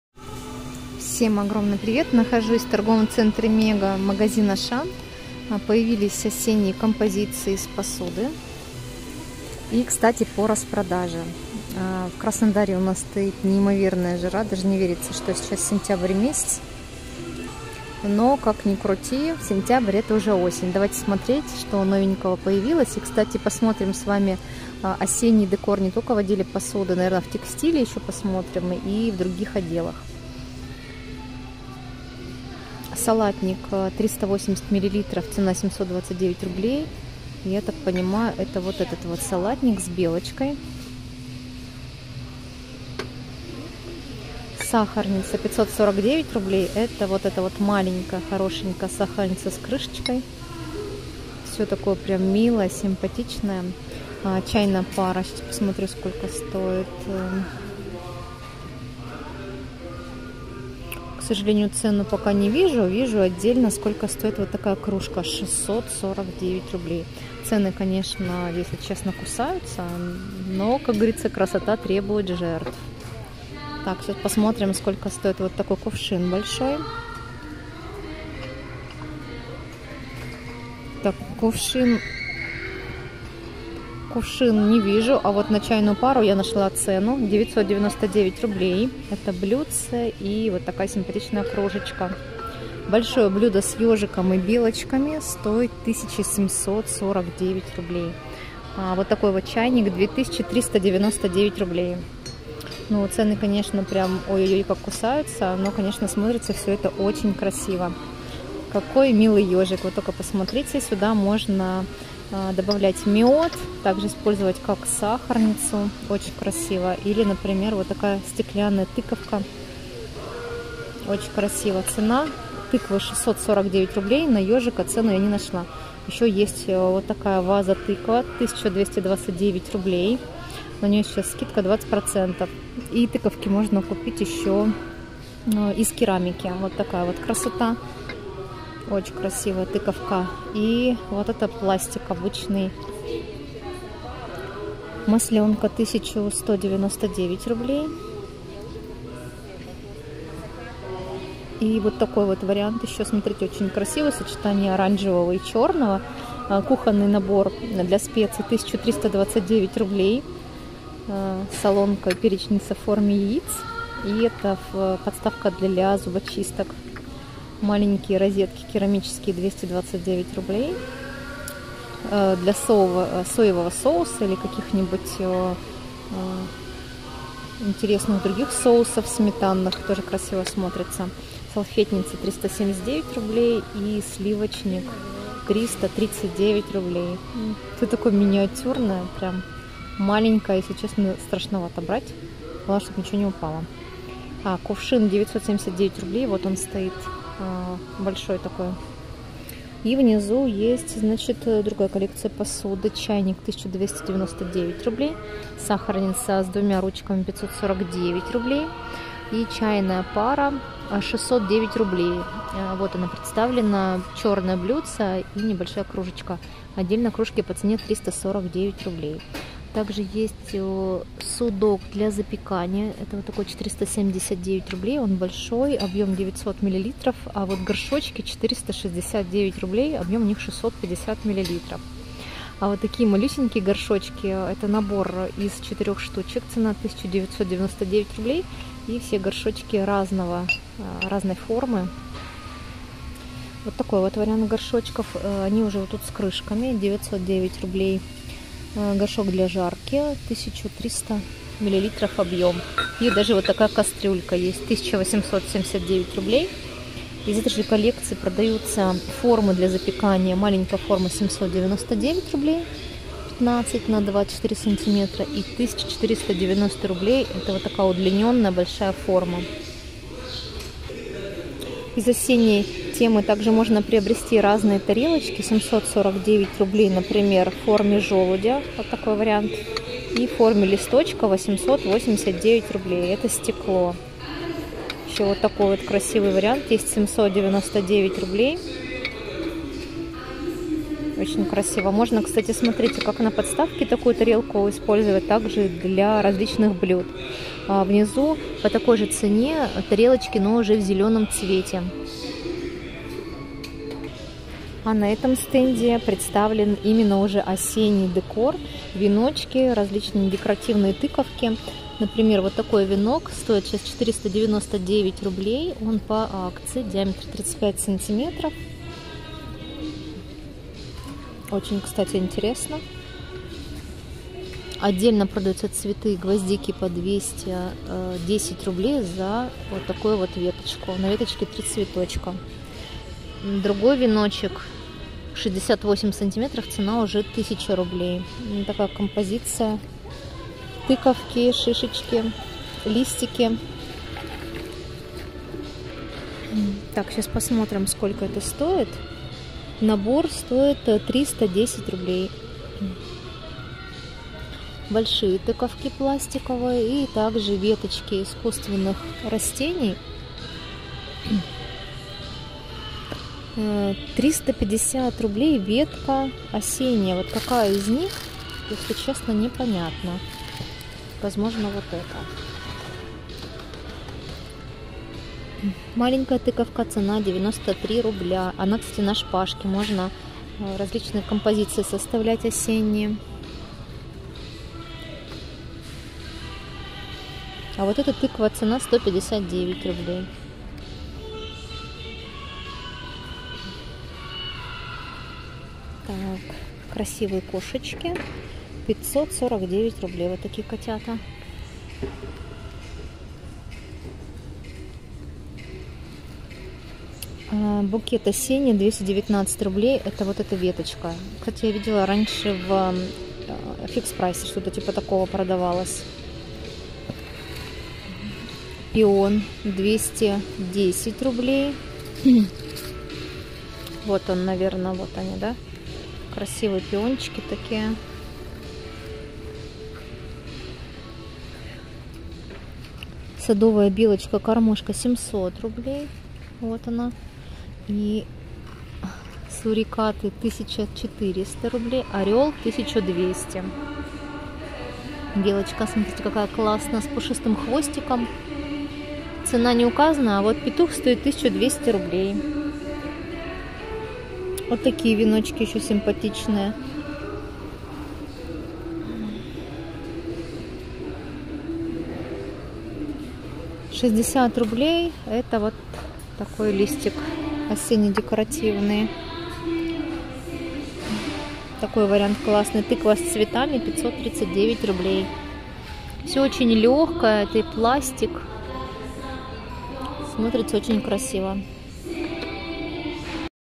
АШАН яркие Осенние НОВИНКИ Посуда+Декор сентябрь 2024
Всем привет! 👋Меня зовут Элла.  В этом видео обзор из  магазина Ашан ТЦ Мега, ул Тургеневское шоссе, Новая Адыгея